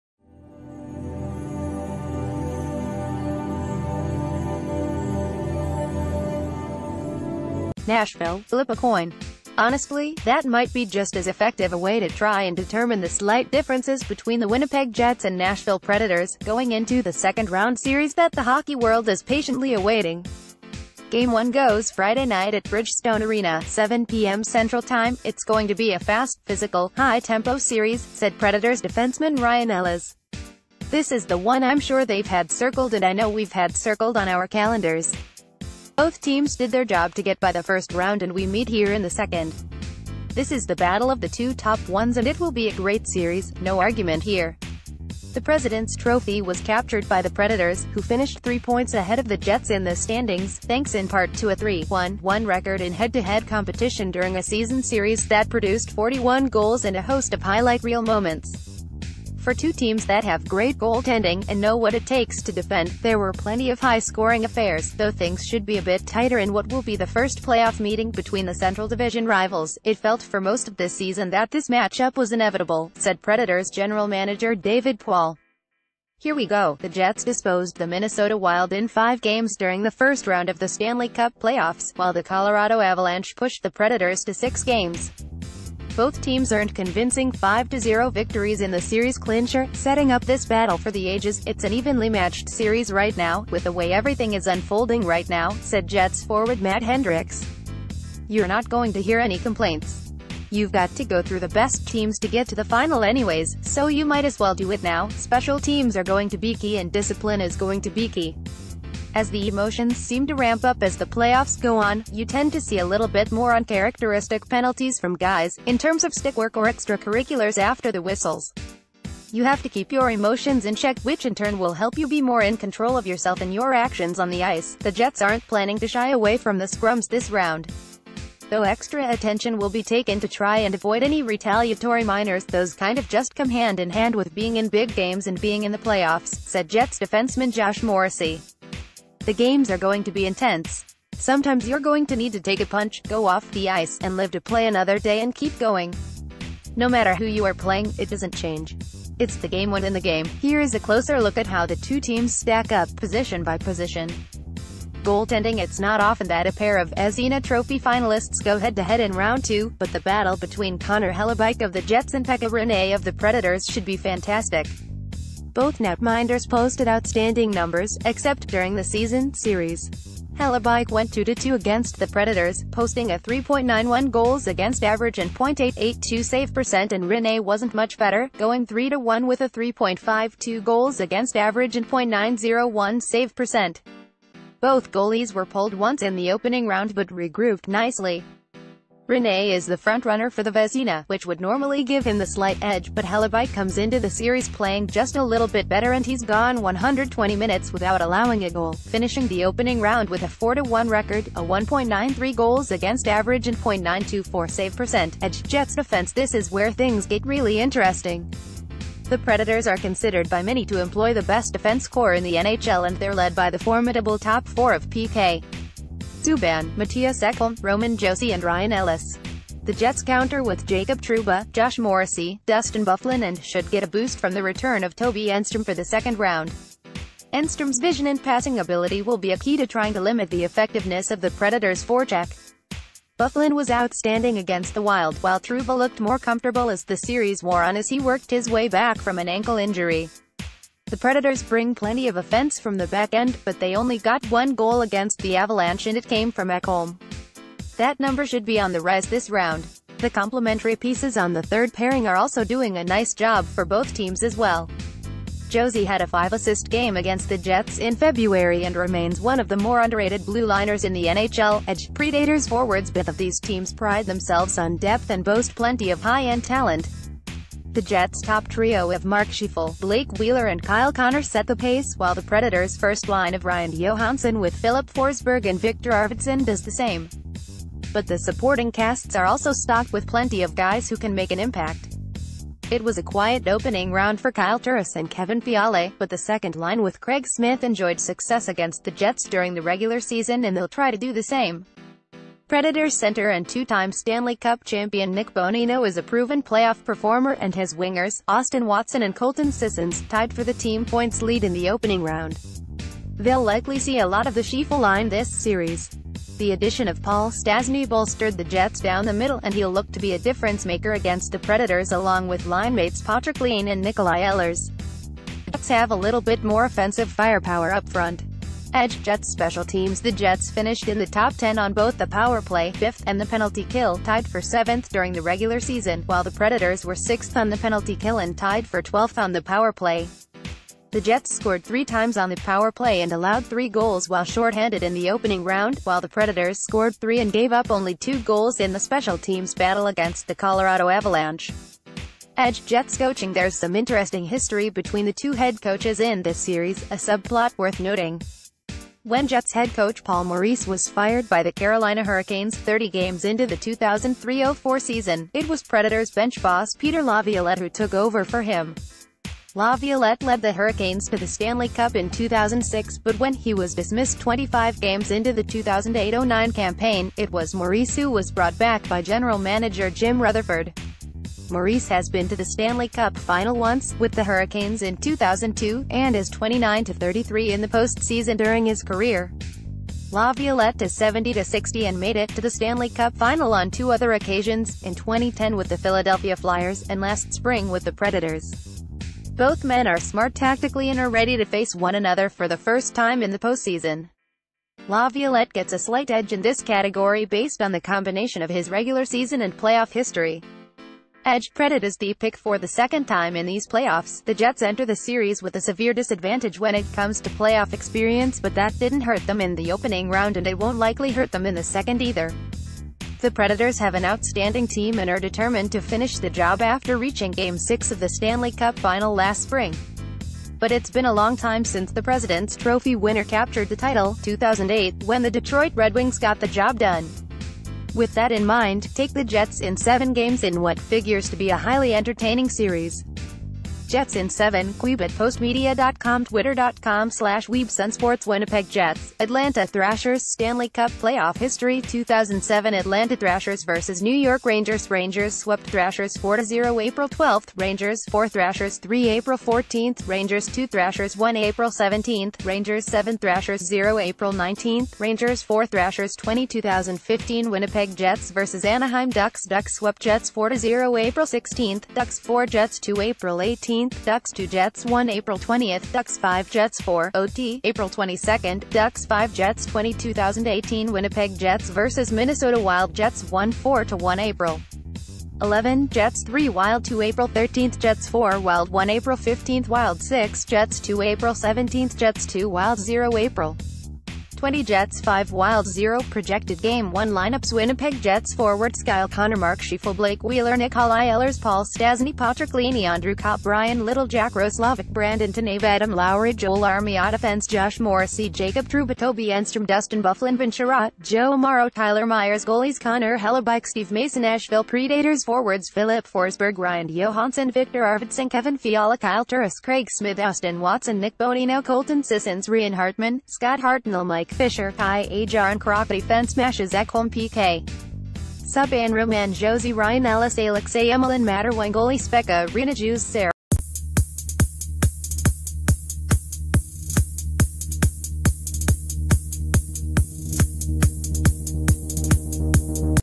Breaking News | Marquee match-up has arrived: Not much to choose between Jets and Predators in seco
Breaking News | Marquee match-up has arrived: Not much to choose between Jets and Predators in second-round slugfest

NASHVILLE – Flip a coin. Honestly, that might be just as effective a way to try and determine the slight differences between the Winnipeg Jets and Nashville Predators, going into the second-round series that the hockey world is patiently awaiting. Game 1 goes Friday night at Bridgestone Arena (7 pm CT).“It’s going to be a fast, physical, high-tempo series,” said Predators defenceman Ryan Ellis. “This is the one I’m sure they’ve had circled and I know we’ve had circled on our calendars. Both team...